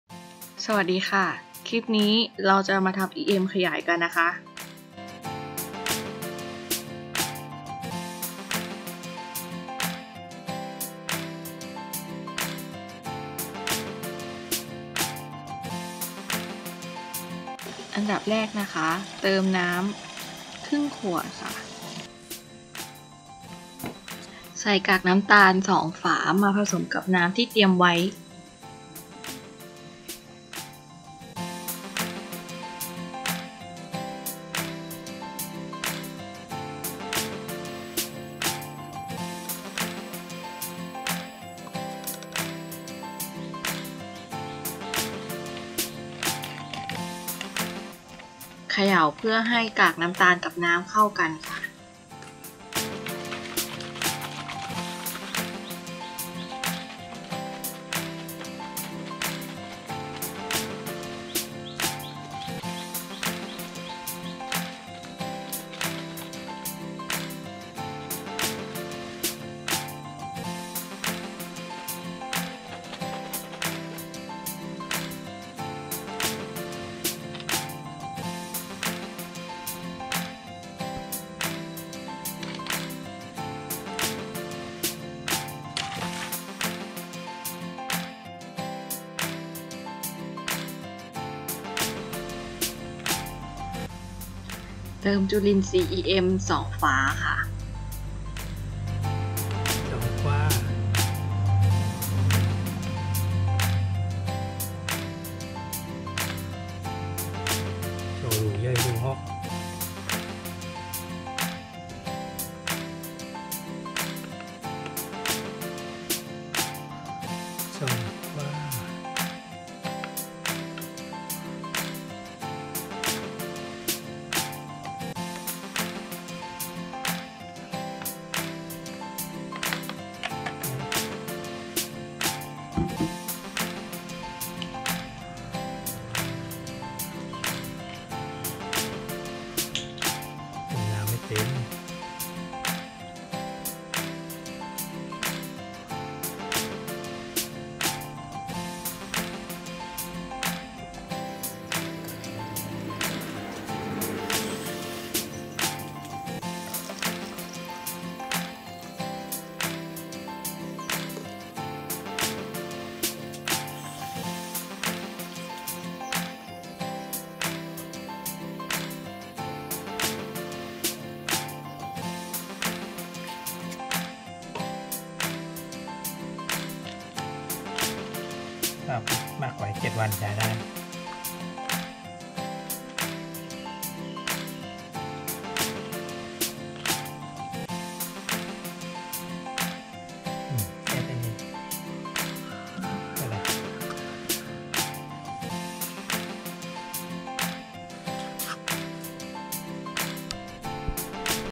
วิธีทำ EM ขยาย ที่ใครก็พอจะรู้ว่าEMมีประโยชน์
มาตราส่วน
น้ำ 1.5 L
EM 2 ช้อนโต๊ะ(ฝา)
กากน้ำตาล 2 ช้อนโต๊ะ
EM มีประโยชน์อะไรบ้าง
ใส่รดน้ำต้นไม้ ใส่ให้สัตว์กิน ใส่ในห้องน้ำที่มีกลิ่นแรง ใส่เพื่อย่อยปุ๋ย
ย่อยอินทรีย์วัตถุ ใส่บำบัดน้ำเสีย ฯลฯ